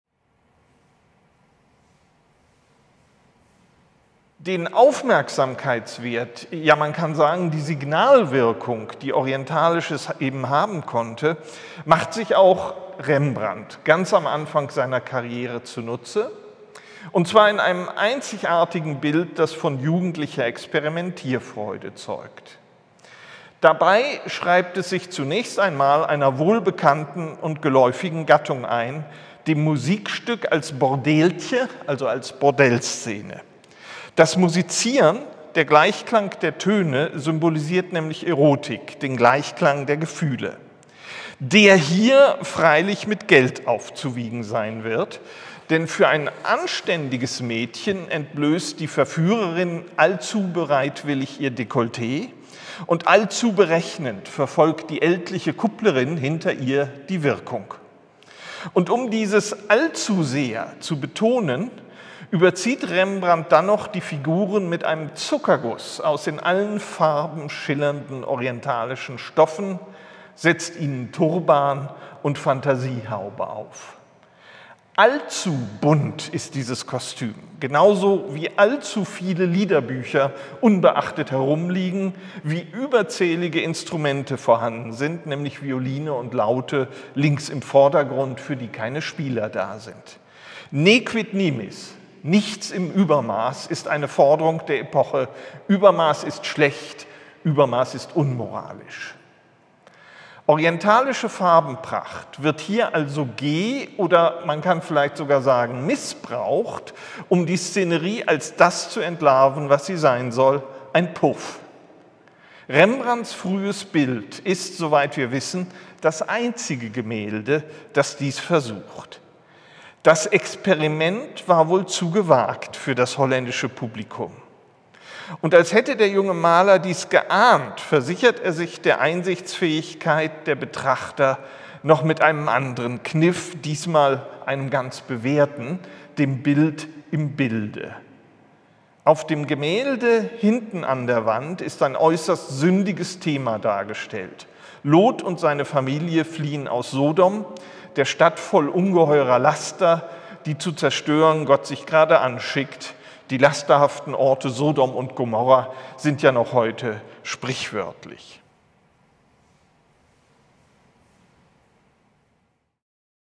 Rembrandt's Orient – West Meets East in Dutch Art of the 17th Century / Highlights (1/3)
The exhibition “Rembrandt's Orient”, which just closed at Kunstmuseum Basel and will be on view soon at Museum Barberini in Potsdam, examines how the painters of the Dutch Golden Age reacted to the areas of the Near, Middle and Far East that had come into view through trade, travel and publications in the 17th century. Curated by Bodo Brinkmann, Gabriel Dette and Gary Schwartz, the exhibition features works by Rembrandt and his colleagues and students, and also includes publications and other sources that illustrate the contemporary vision of the Orient.

In three short videos the curators Bodo Brinkmann and Gabriel Dette present highlights of the show. In the first video, Dr Bodo Brinkmann (Curator, Old Masters, 15th-18th Centuries at Kunstmuseum Basel) explains what Rembrandt van Rijn's painting “Musical Company” has to do with Sodom and Gomorrah. (in German with english subtitles).

Rembrandt's Orient – West Meets East in Dutch Art of the 17th Century at Kunstmuseum Basel | Neubau. Highlights (1/3), February 3, 2021.